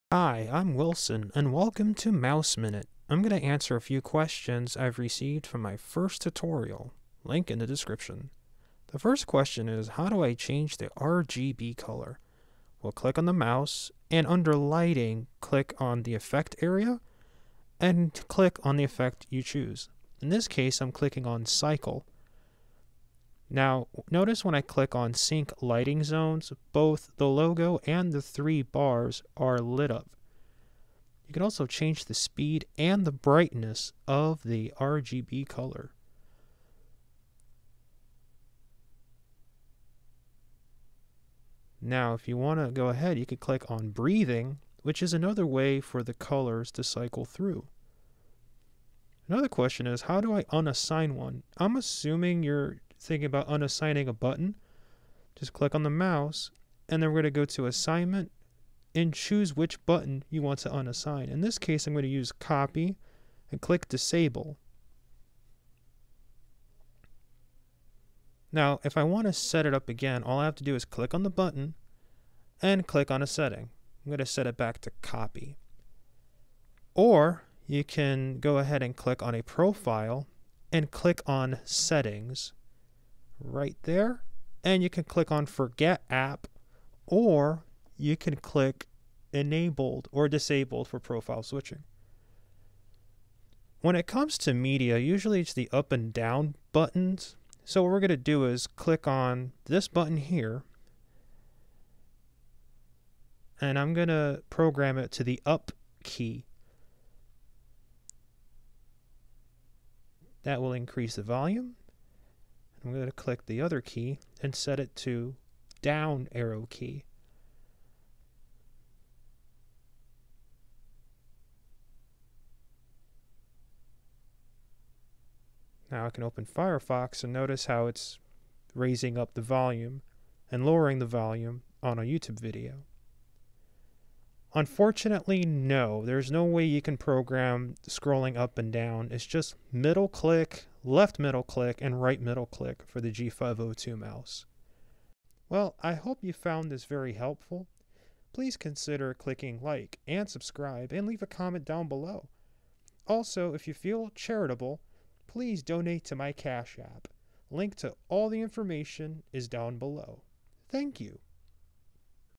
How to Program Logitech G502 Hero Buttons FAQ 1
In the this first episode of Mouse Minute I wanted to answer some FAQs  users have about the Logitech G502.